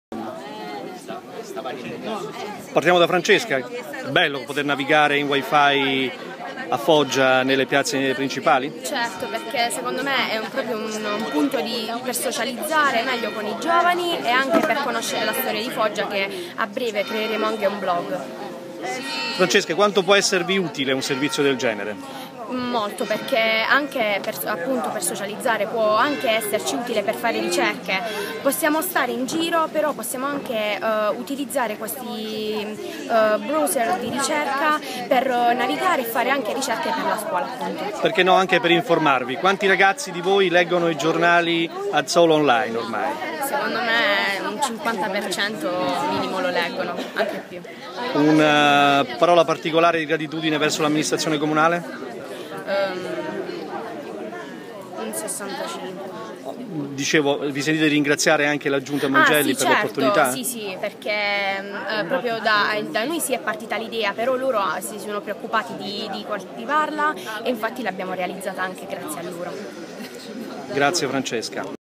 Videointervista a Francesca Laquaglia, assessore junior all
L'importanza del Wi-Fi per i giovani foggiani